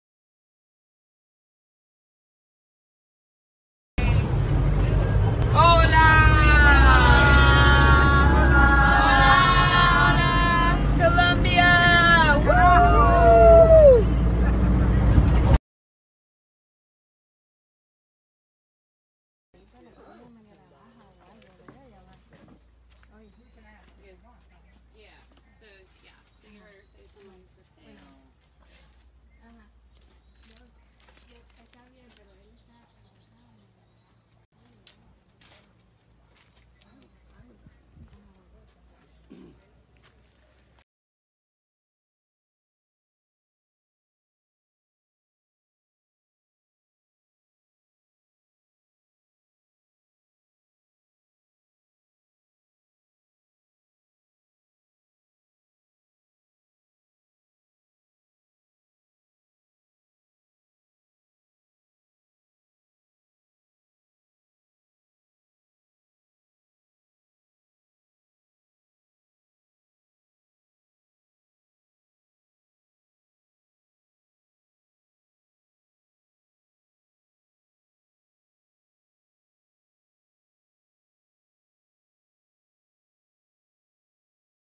Bods Abroad: Conflict Resolution & Trauma in Colombia
Washburn University student Molly Manske shares a glimpse of her study abroad trip to Colombia in 2019. Learn more about Washburn's study abroad program and apply at washburn.studioabroad.com.